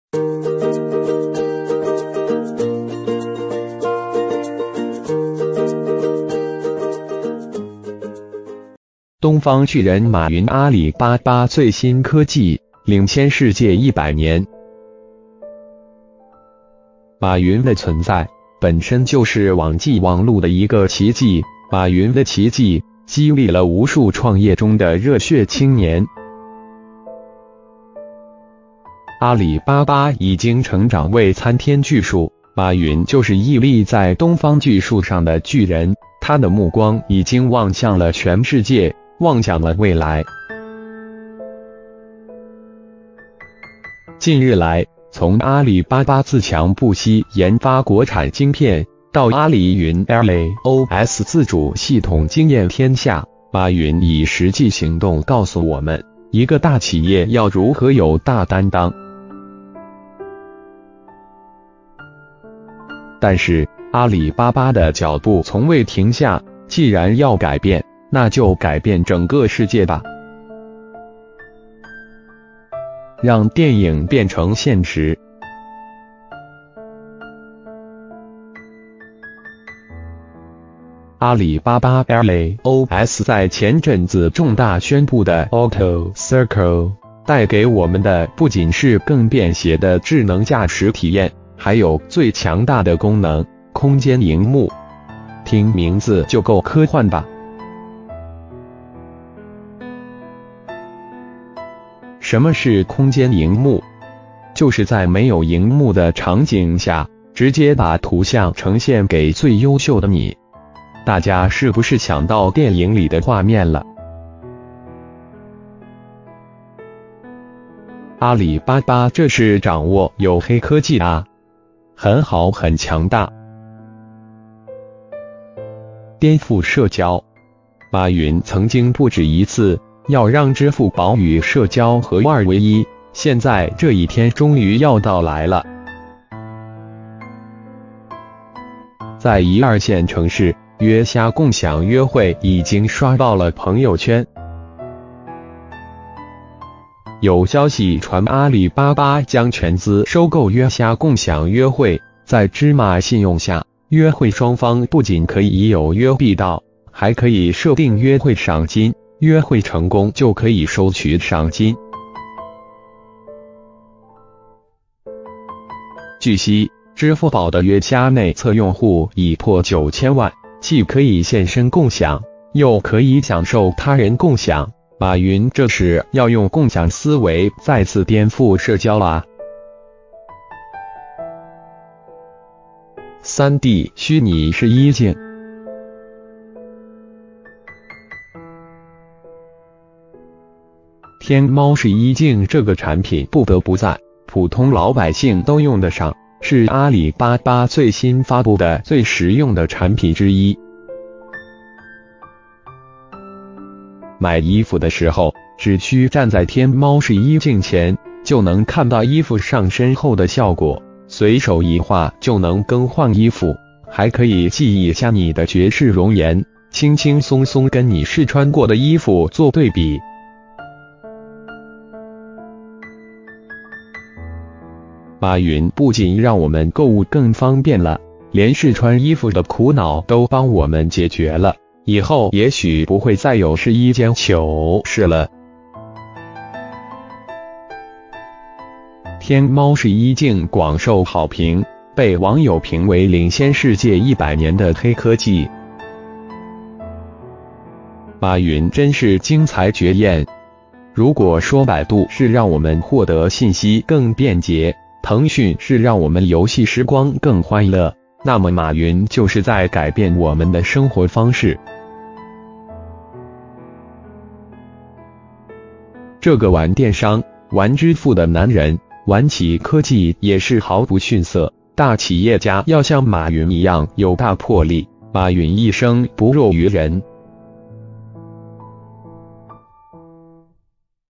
東方巨人馬雲阿里巴巴最新科技，領先世界100年
馬雲的存在，本身就是網際網路的一個奇蹟，馬雲的奇蹟，激勵了無數創業中的熱血青年。

阿里巴巴已經成長為參天巨樹，馬雲就是屹立在東方巨樹上的巨人，他的目光已經望向了全世界，望向了未來！近日來，從阿里巴巴自強不息研發國產晶片，到阿里雲AliOS自主系統驚艷天下，馬雲以實際行動告訴我們，一個大企業要如何有大擔當！

但是，阿里巴巴的腳步從未停下，既然要改變，那就改變整個世界吧！讓電影變成現實阿里巴巴AliOS在前陣子重大宣布的「Auto Circle」，帶給我們的不僅是更便攜的智能駕駛體驗，還有最強大的功能——空間螢幕！聽名字就夠科幻吧！

什麼是空間螢幕？就是在沒有螢幕的場景下，直接把圖像呈現給最優秀的你！大家是不是想到電影里的畫面了？阿里巴巴這是掌握有黑科技啊！很好很強大！顛覆社交馬雲曾經不止一次，要讓支付寶與社交合二為一，現在這一天終於要到來了！在一二線城市，約蝦共享約會已經刷爆了朋友圈。有消息傳阿里巴巴將全資收購約蝦共享約會，在芝麻信用下，約會雙方不僅可以有約必到，還可以設定約會賞金，約會成功就可以收取賞金。

據悉，支